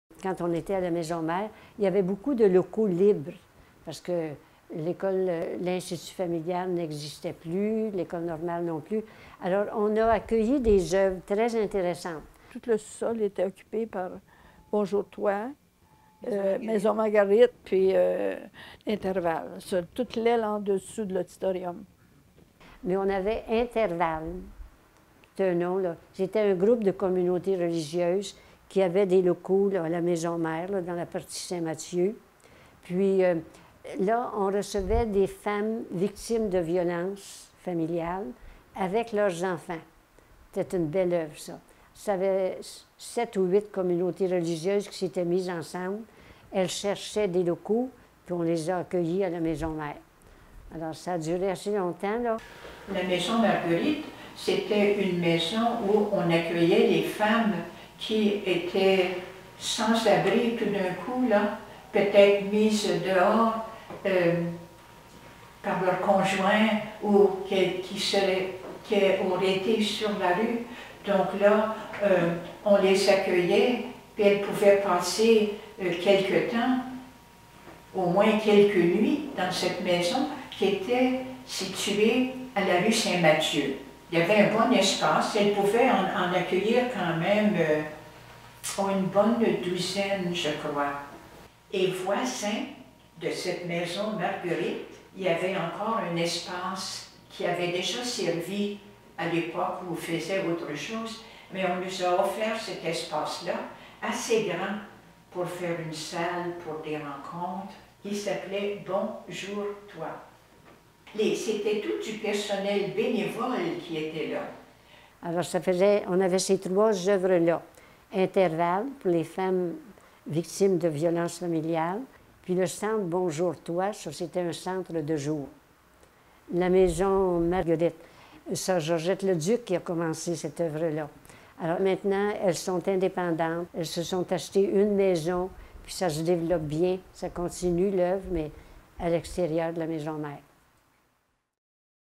Les organismes de bienfaisance à la maison mère des Sœurs Grises
Écoutez des sœurs décrire certaines des œuvres dont la maison mère accueillait les activités au milieu du 20e siècle, gage du dévouement de l’ordre à l’égard des personnes vulnérables.